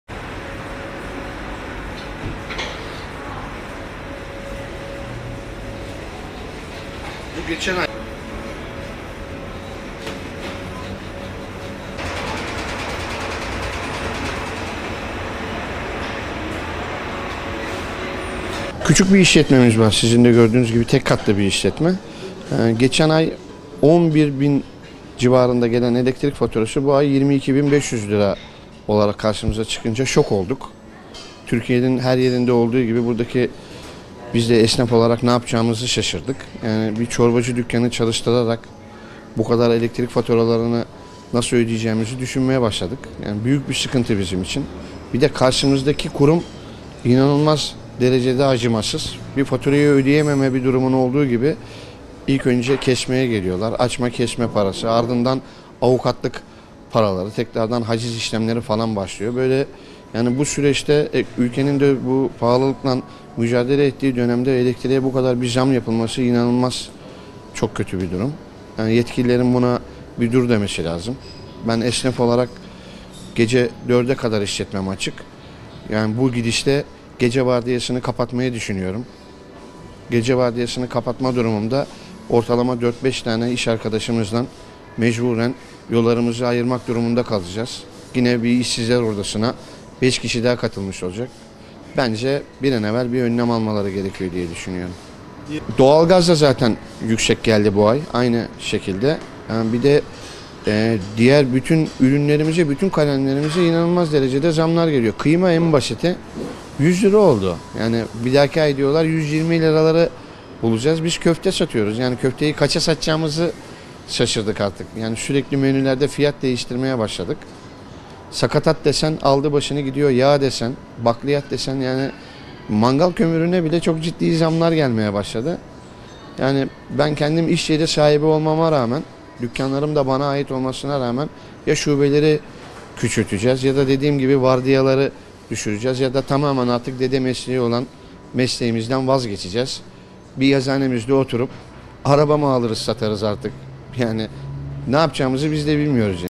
"Elektrik faturası bu ay 22 bin 500 gelince şok olduk"
Tekirdağ’da 22 bin 500 lira elektrik faturası gelen restoran işletmecisi Volkan Nallar, “Geçen ay 11 bin gelen elektrik faturası bu ay 22 bin 500 gelince şok olduk. Türkiye’nin her yerinde olduğu gibi biz de esnaf olarak ne yapacağımızı şaşırdık. Çorba dükkanı çalıştırarak bu elektrik faturalarını nasıl ödeyeceğimizi düşünmeye başladık. Ya şubeleri küçülteceğiz ya vardiyaları düşüreceğiz ya tamamen, dede mesleği olan mesleğimizden vazgeçeceğiz” dedi.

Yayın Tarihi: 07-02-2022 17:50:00